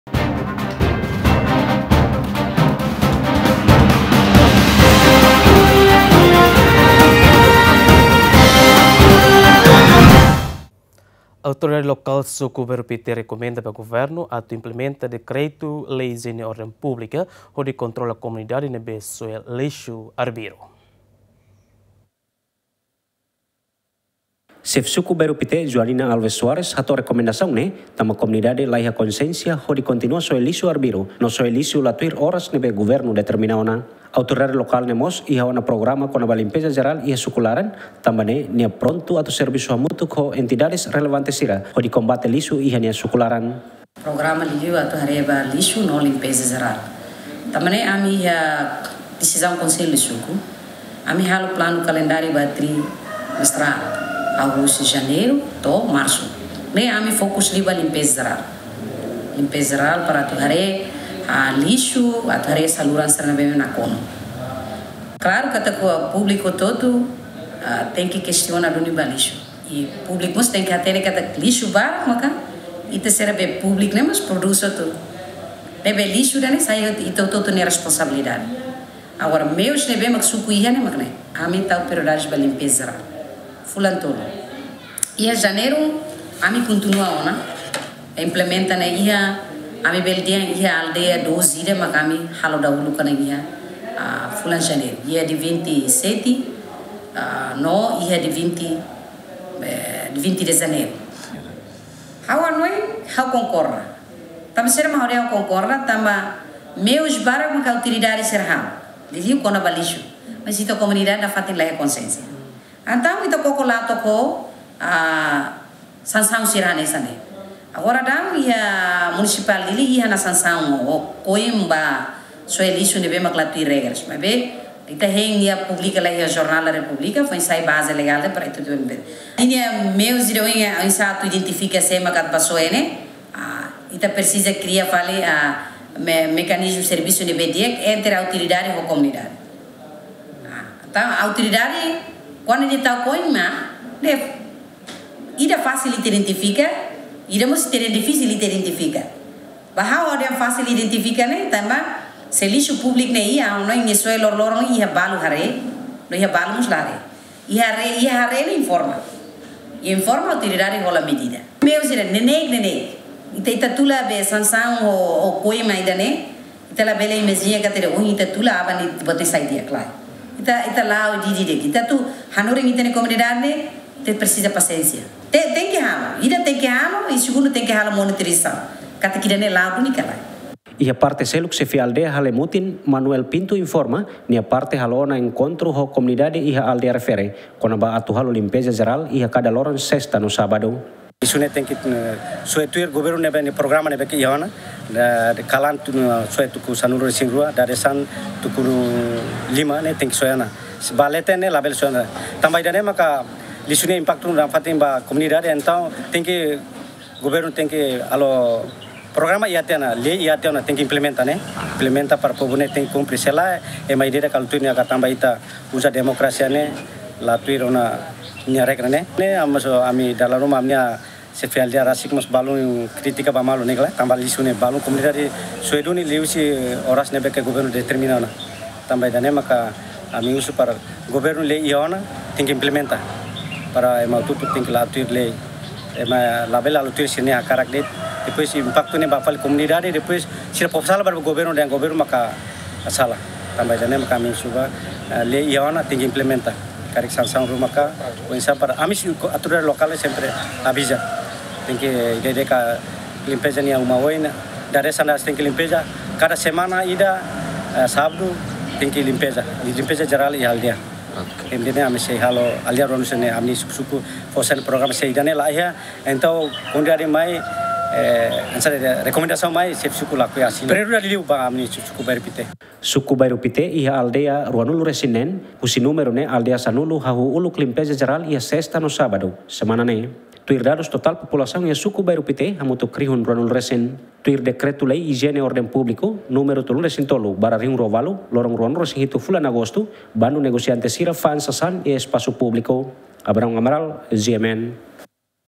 Autoridade Suku Bairo-Pite rekomenda ba Governu atu implementa dekreitu lei hijiene ordem públika
Autoridade lokal Suku Bairo-Pite rekomenda ba Governu atu implementa dekreitu lei hijiene ordem públika hodi kontrola komunidade ne’ebé soe lixu arbiru